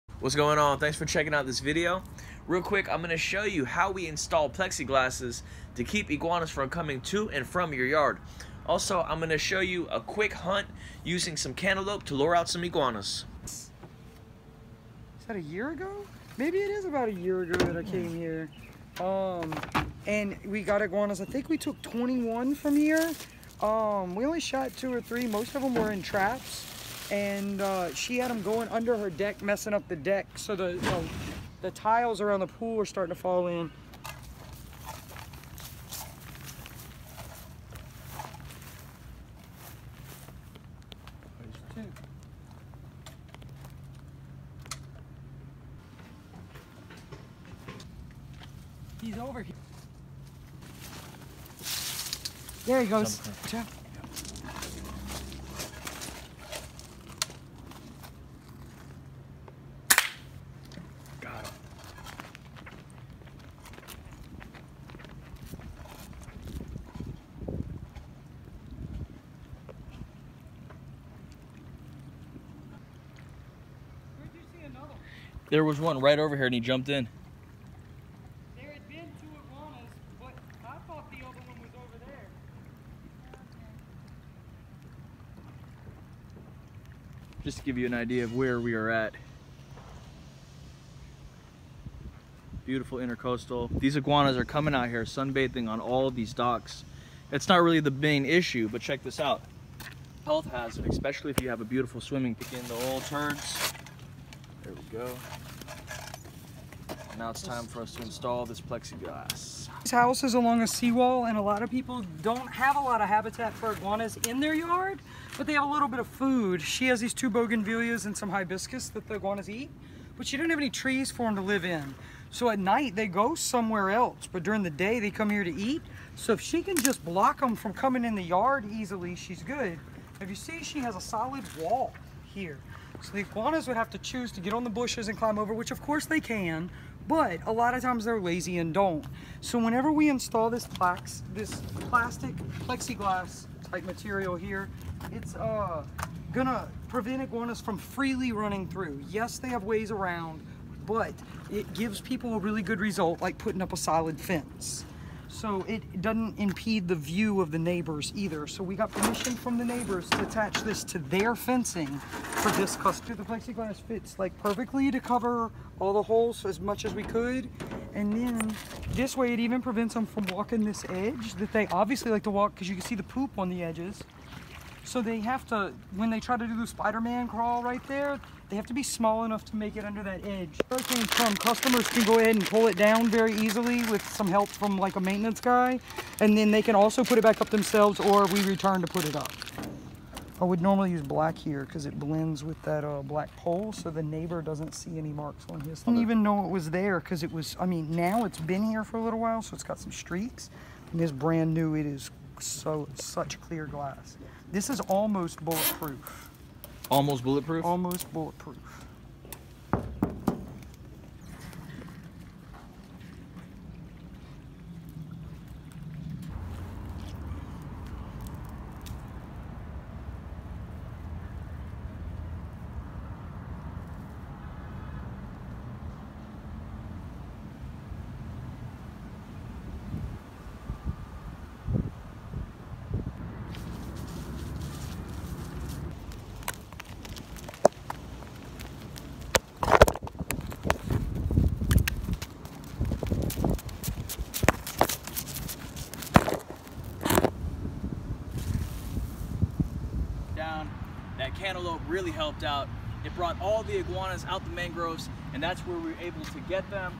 Installing plexiglass iguana Fence using cantaloupe to Lure Iguanas
Iguana iguana hahha